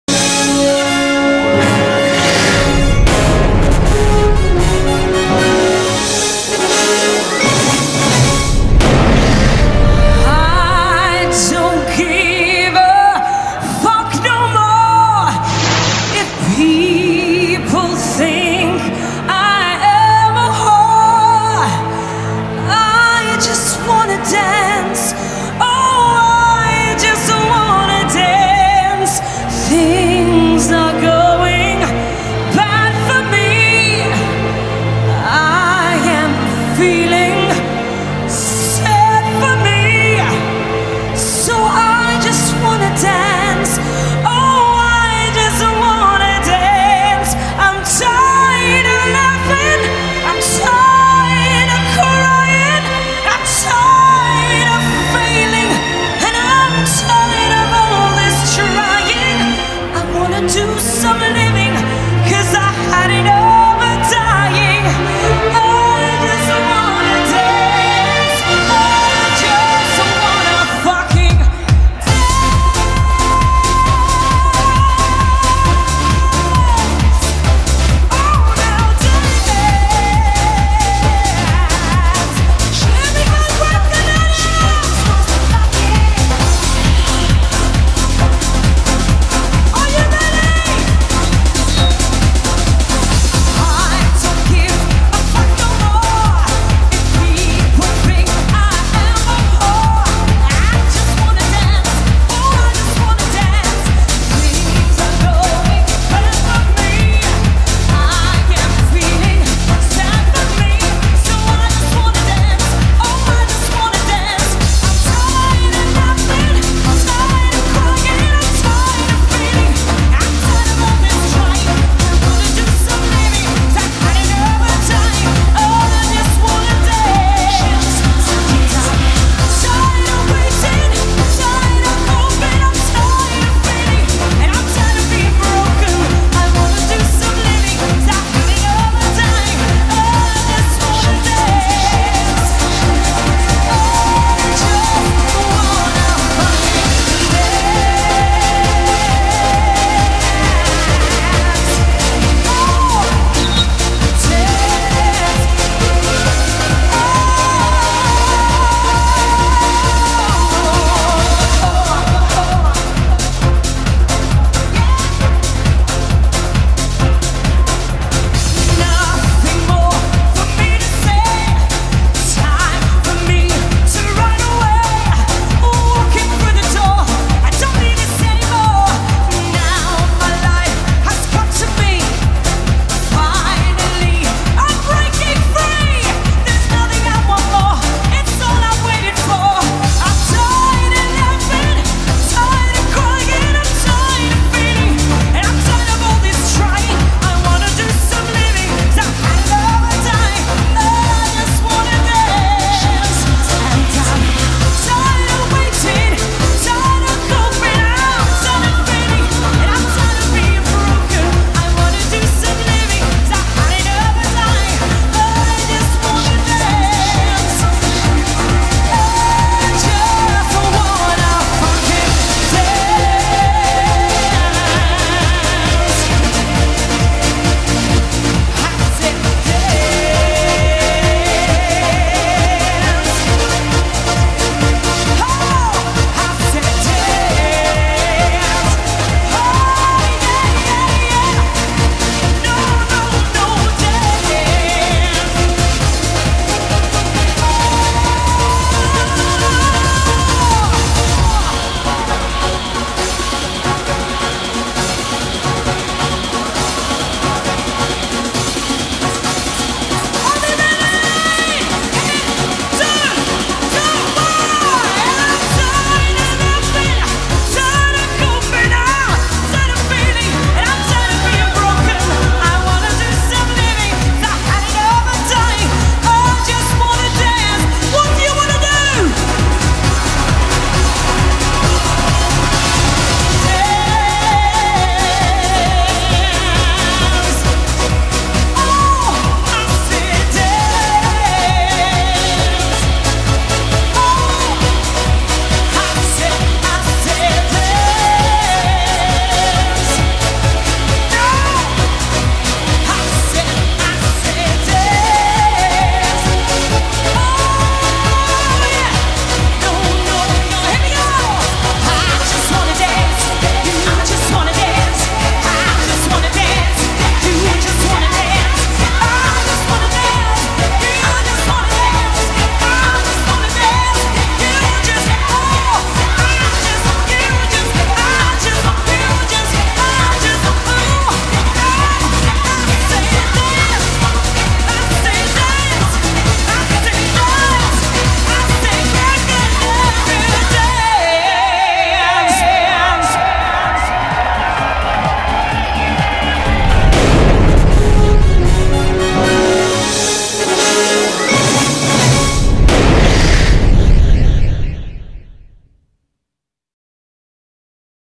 Eva Iglesias (Eva la Diva) Maspalomas Pride 2012
The fabulous Eva Iglesias aka Eva la Diva at Maspalomas Pride 2012. Taken from our official DVDs of Maspalomas Pride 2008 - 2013